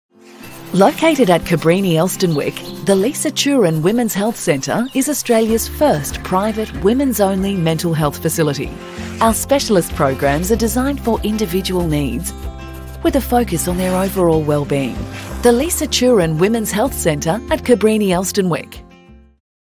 Cabrini Women's Mental Health | Lisa Thurin Women's Health Centre, Cabrini Elsternwick
Australia's first private women's only mental health facility, our specialist programs are designed for individual needs, with a focus on our clients' overall wellbeing.